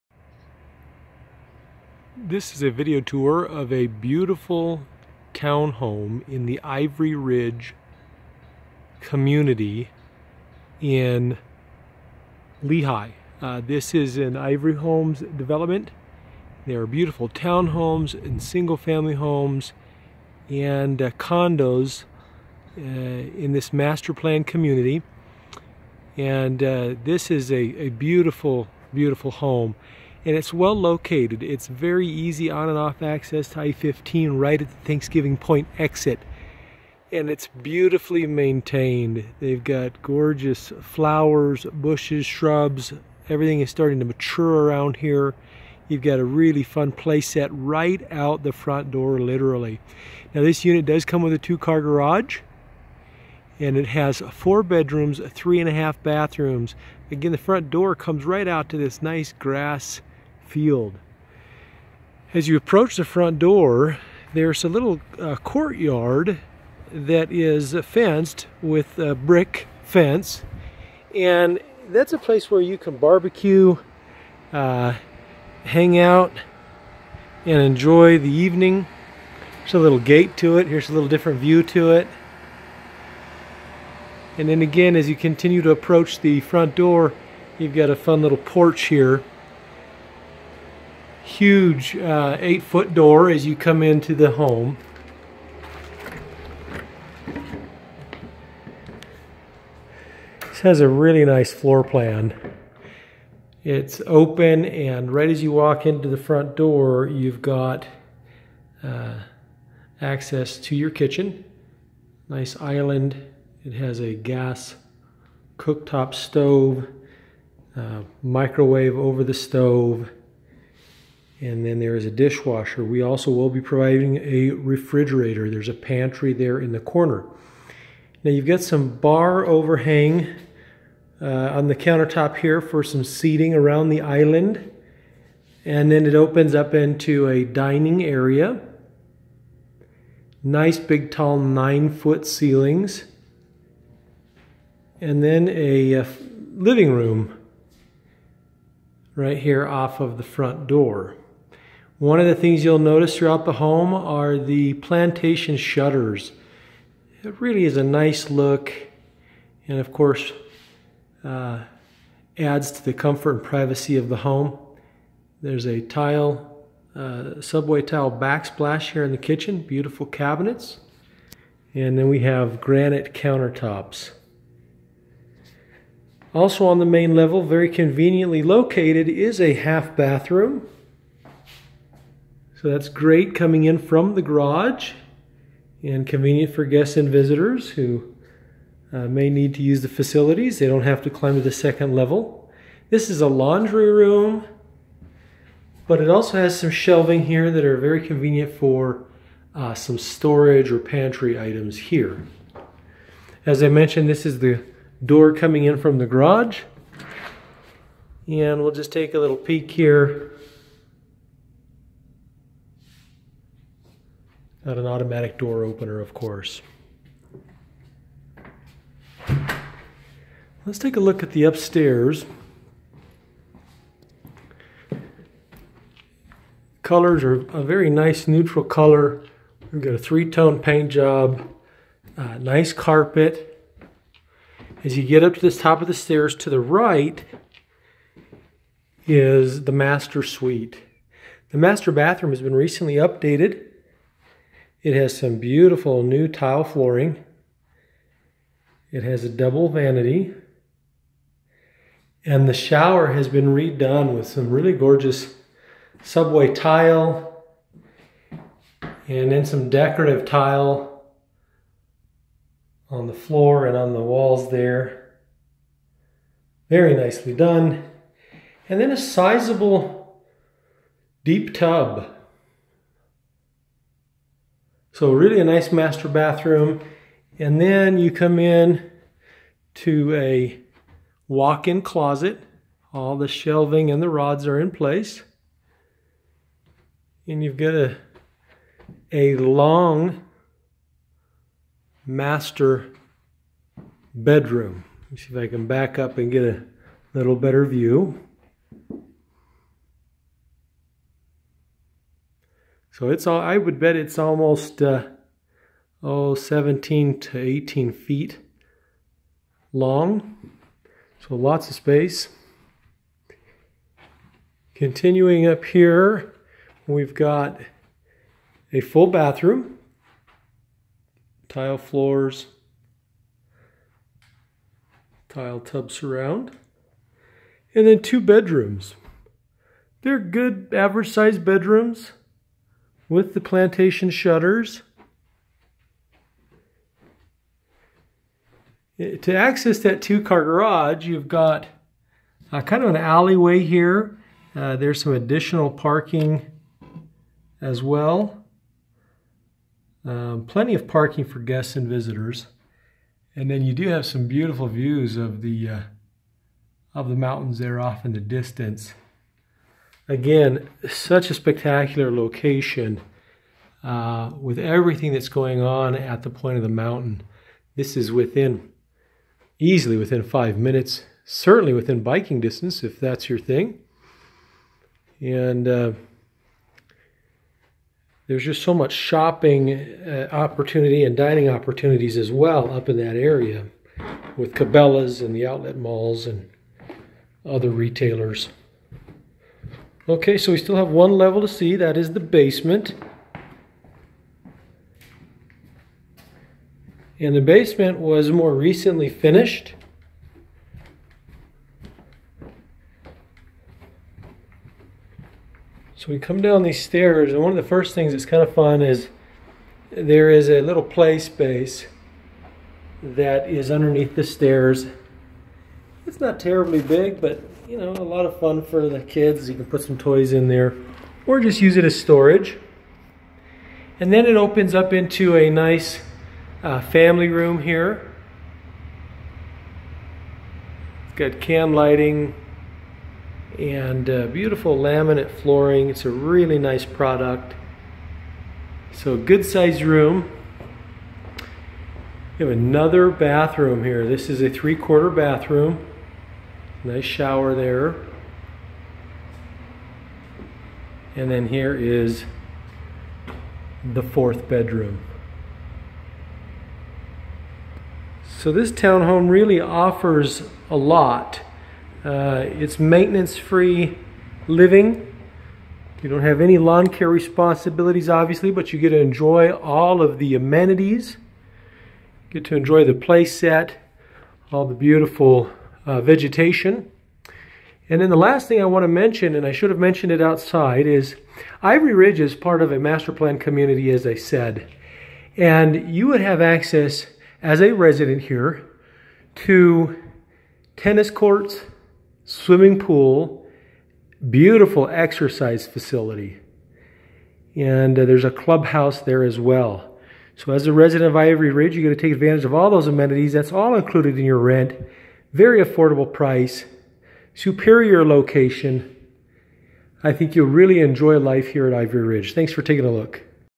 For Rent - 135 W 3300 N Lehi, UT
Incredible town home in the Ivory Ridge Development located just a few minutes from point of the mountain / Silicone Slopes in Lehi, UT.  Fully finished with 4 bedrooms and 3.5 bathrooms.  Great condition.  The community offers access to pool, work out facility, clubhouse, and tennis courts.  This will go fast.   Professionally managed by Home Basics Real Estate